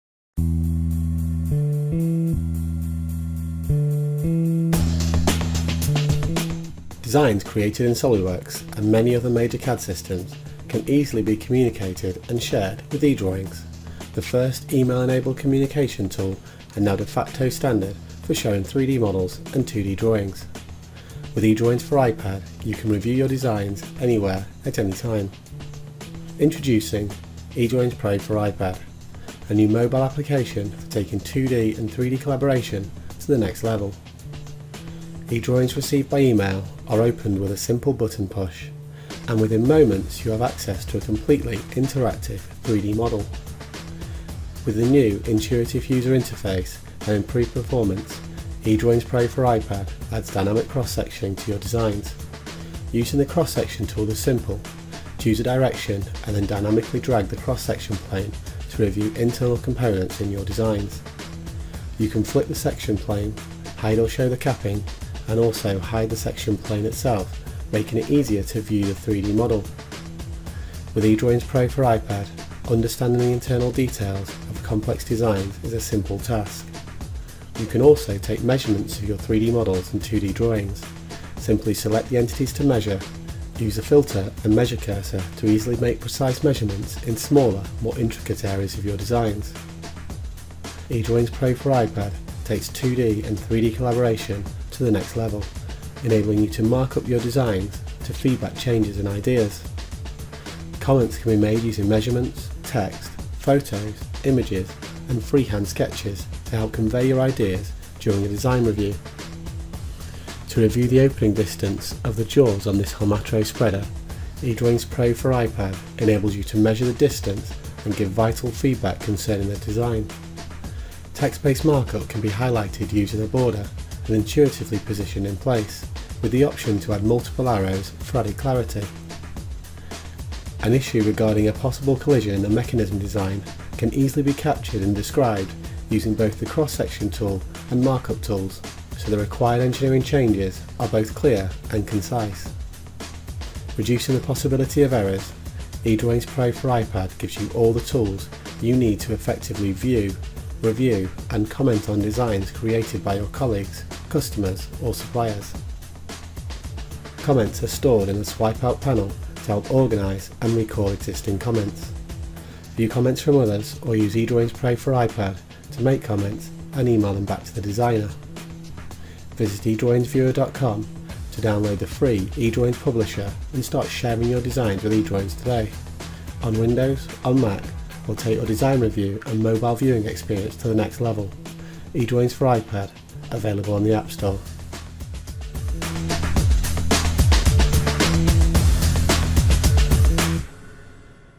eDrawings Pro for iPad: First Look
With eDrawings® Pro for iPad you can create markups on your drawings, including text notes, freehand notations, dimensions, cross-section views, photos, and camera snapshots. You can share your marked up eDrawings files with others for review and further comments. All markups with history are saved within the eDrawings file.